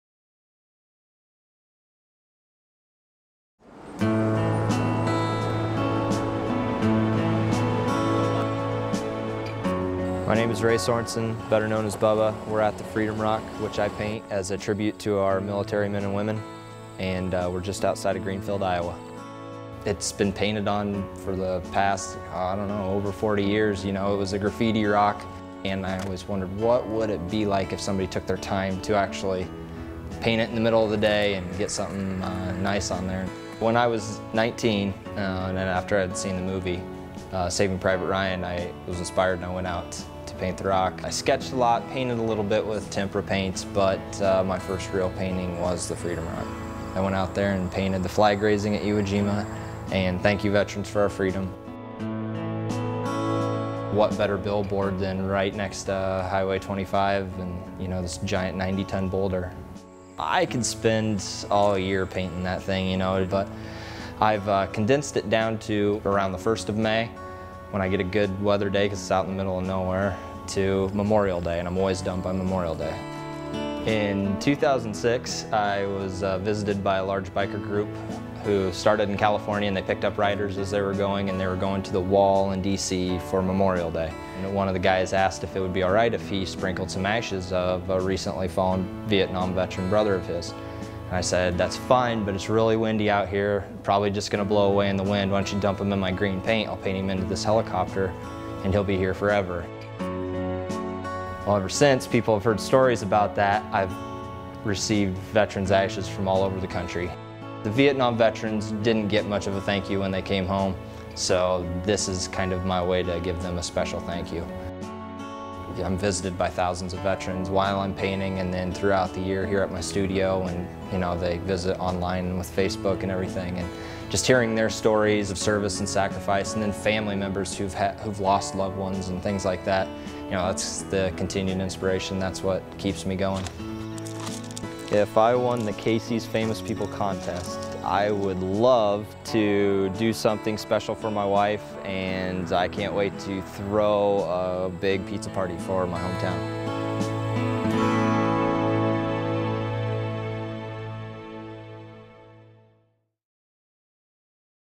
Casey's Famous People: Finalist #1- Freedom Rock Painter
Ray Sorensen of Greenfield, IA is a finalist for our Casey's Famous People contest! Watch his video here, and vote for the $5K winner on our Facebook